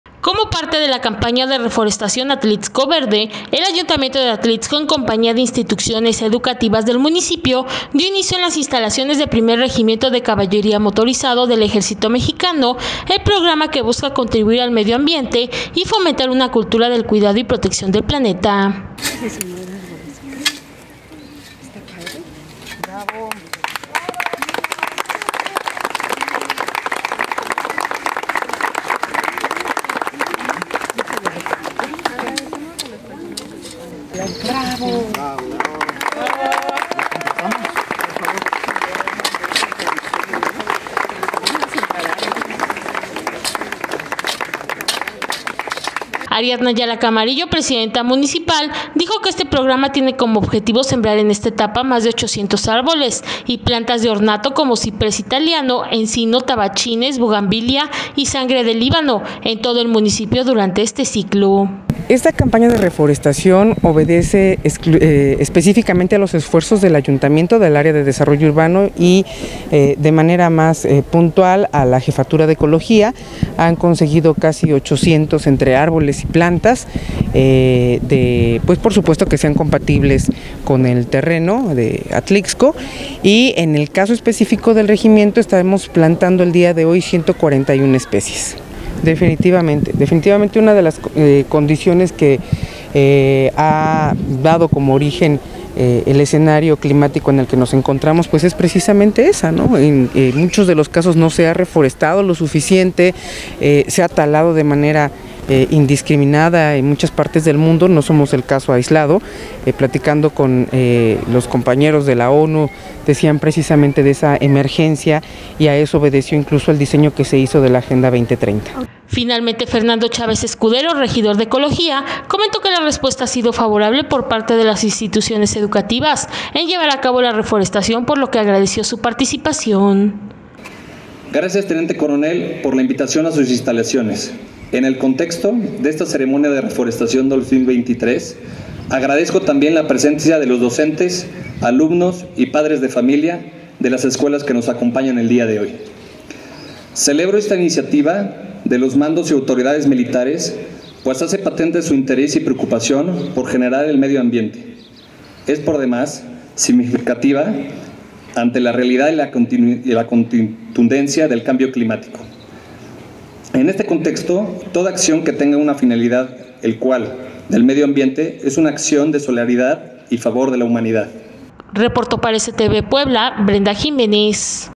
Enterate 🟣| Arranca campaña de reforestación “Atlixco Verde”
Enterate 🟣| Como parte de la campaña de reforestación “Atlixco Verde”, el ayuntamiento de Atlixco, en compañía de instituciones educativas del municipio, dio inicio en las instalaciones del 1er. Regimiento de Caballería Motorizado del Ejército Mexicano, el programa que busca contribuir al medio ambiente y fomentar una cultura del cuidado y protección del planeta.